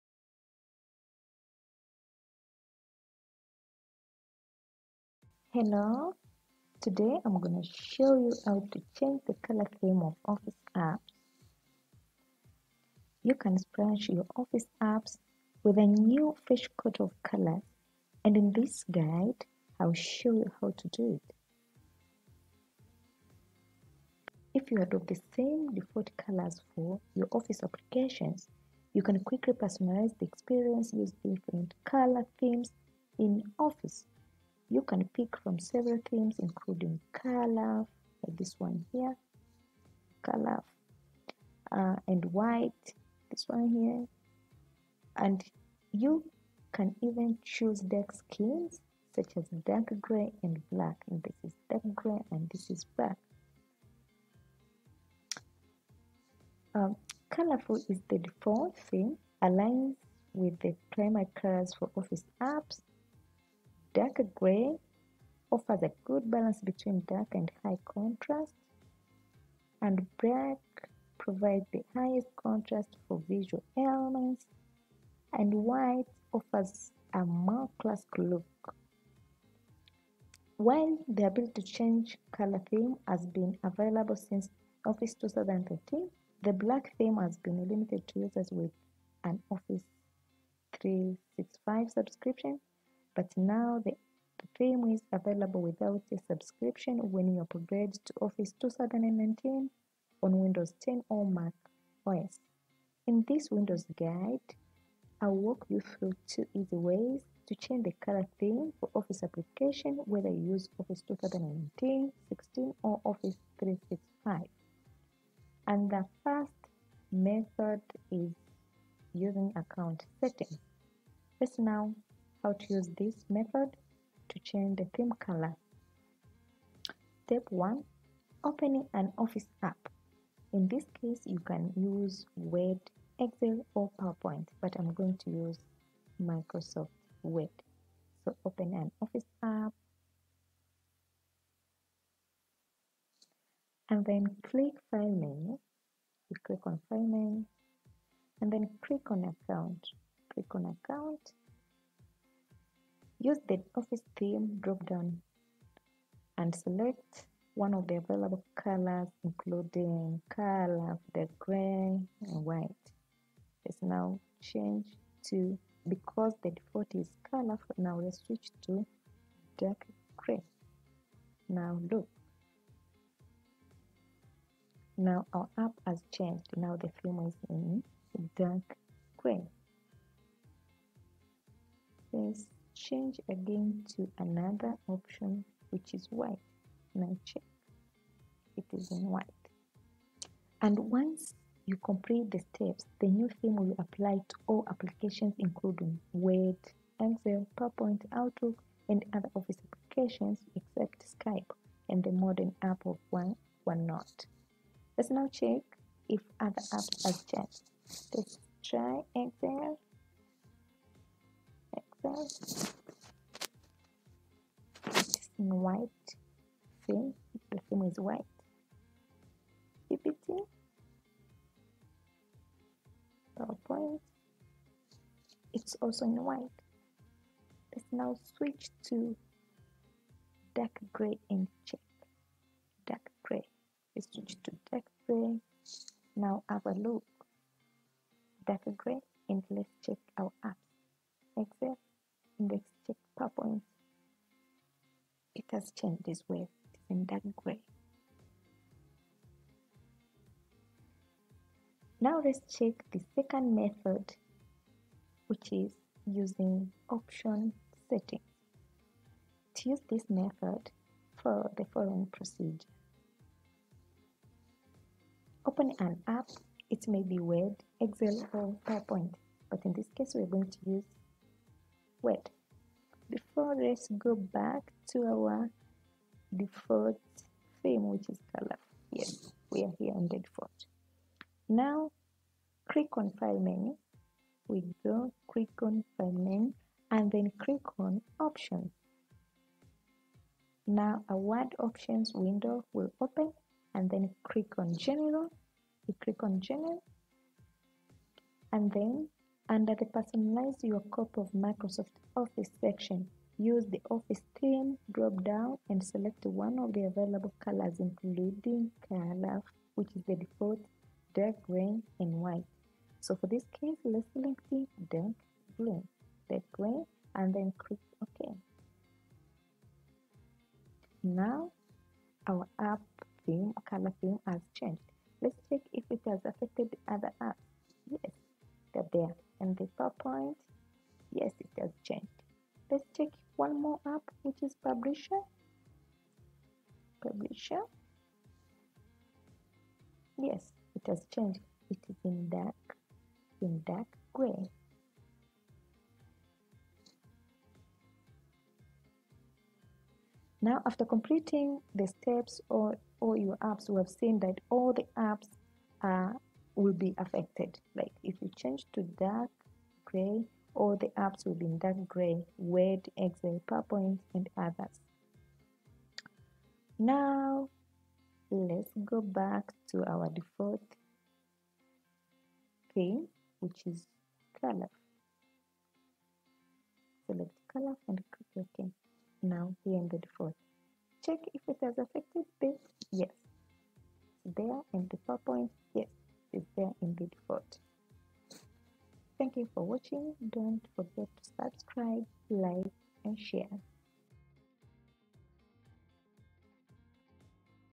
How to change the color theme of Office apps (Ms 2013-2019 &365)
Doc0ument themes make it easy to coordinate colors, fonts, and graphic formatting effects across your Word, Excel, and PowerPoint documents and update them quickly. You can change the entire theme or just customize theme fonts, colors, or effects.